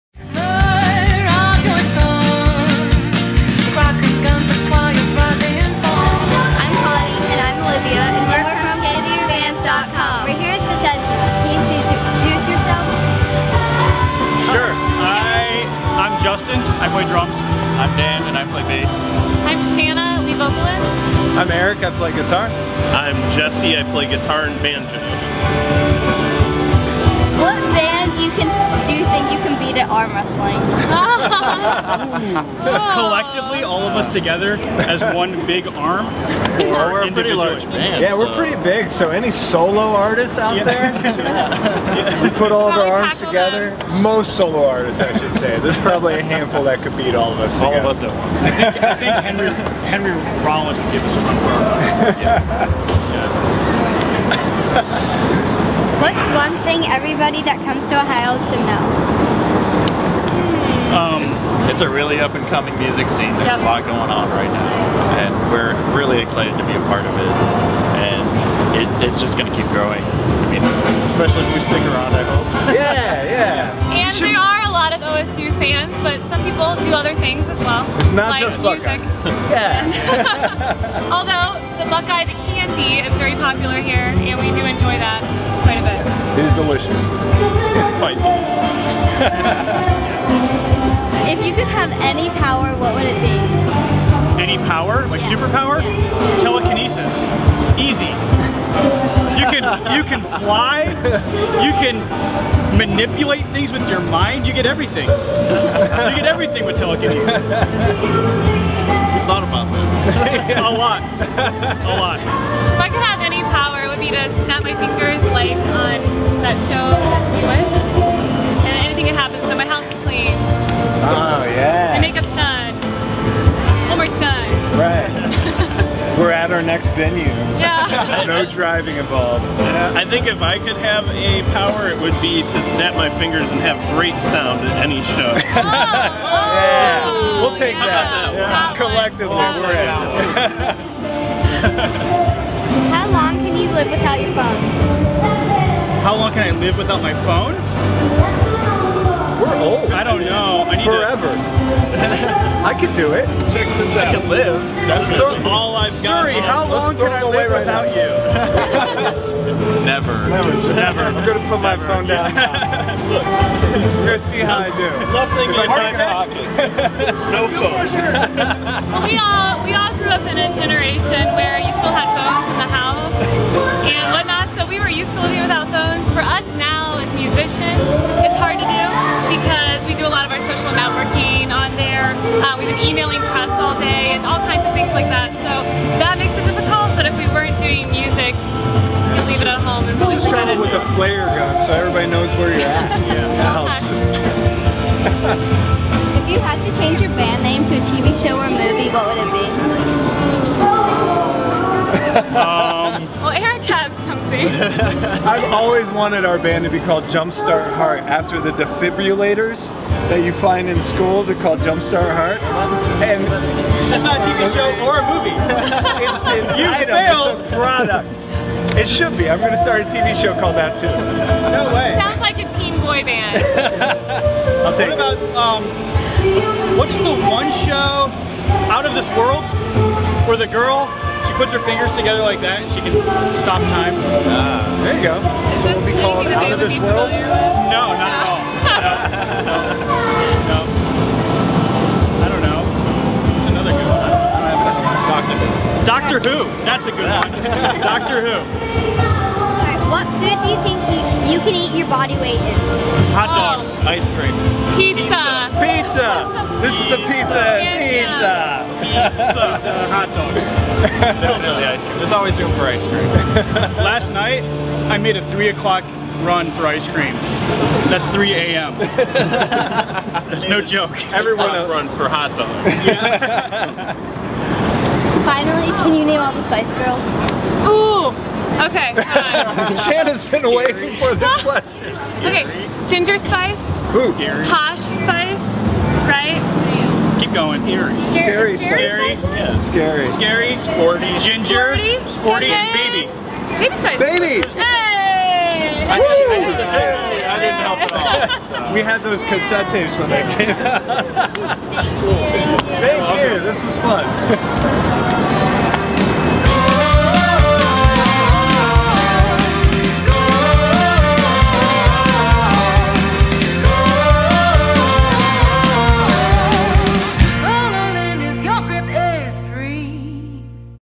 Kids Interview Bands - Bethesda
It was a HOT one when we interviewed Kent, Ohio's Bethesda on the final day of the 2013 Bunbury Music Festival. Turns out, it would be our last interview of the weekend as we hopped into the car and headed back to Columbus rather than stick it out for the rest of the evening. Unfortunately, when doing interviews at festivals it's difficult to find a quiet spot so you'll hear some of Mia Carruthers' set in the background as Connie and Olivia chat with Bethesda.

This interview was filmed on July 14, 2013.